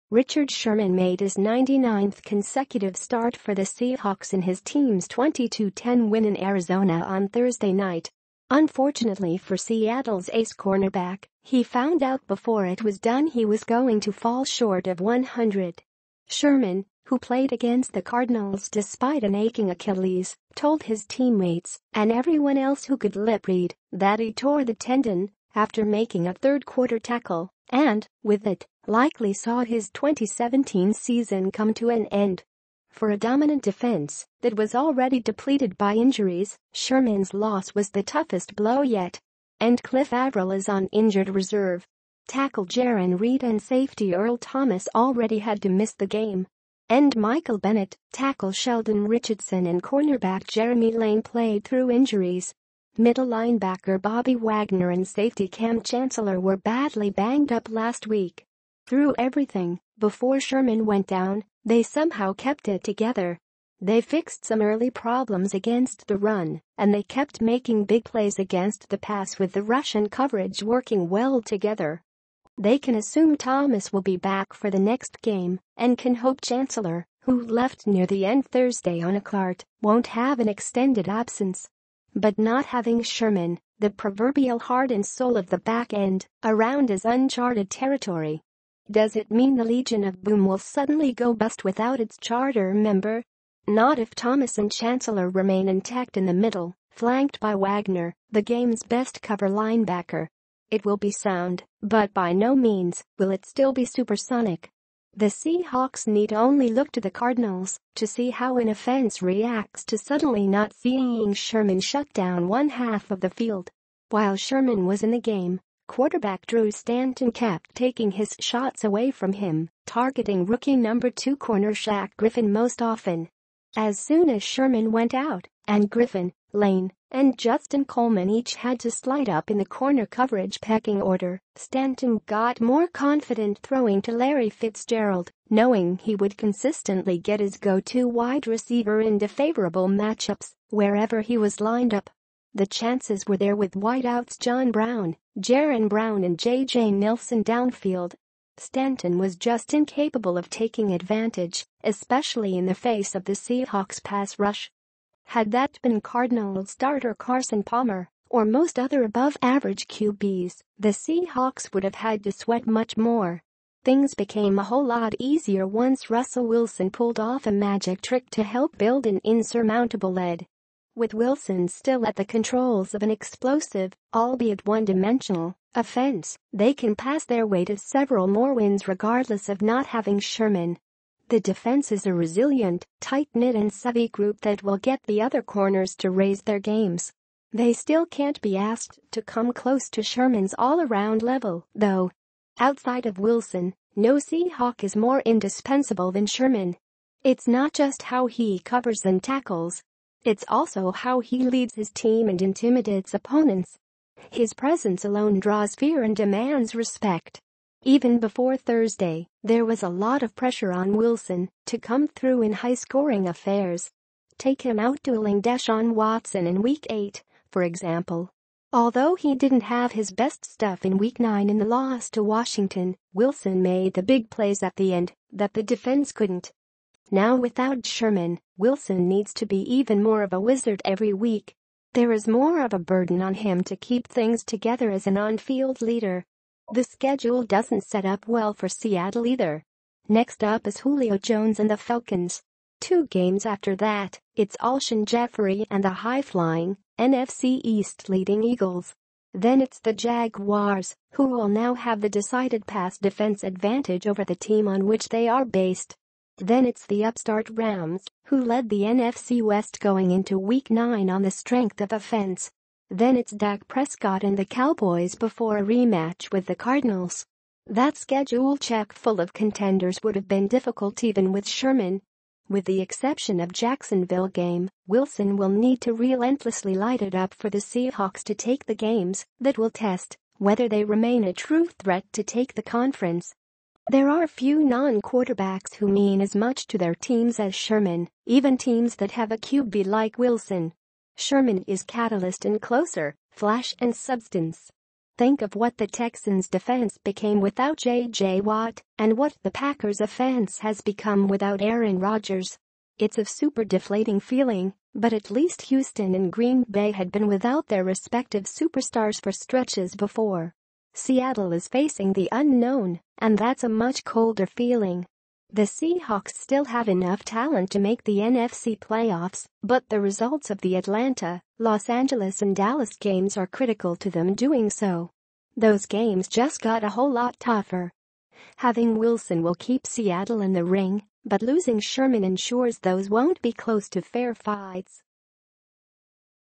Richard Sherman's injury chills Seahawks, sends them down uncertain path
Richard Sherman made his 99th consecutive start for the Seahawks in his team's 22-10 win in Arizona on Thursday night. Unfortunately for Seattle's ace cornerback, he found out before it was done he was going to fall short of 100.